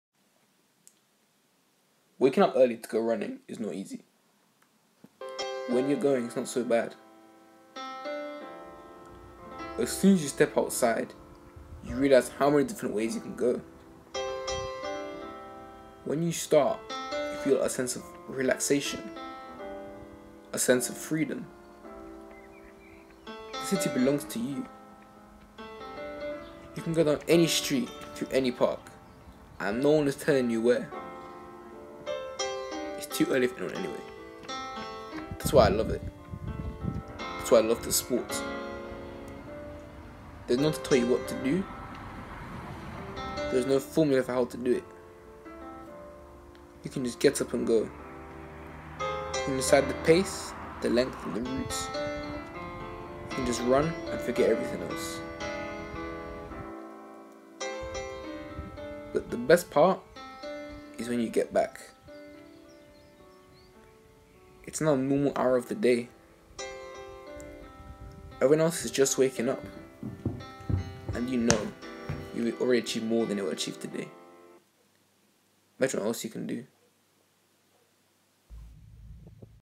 Morning run - Short film
A short film I made about going for a morning run.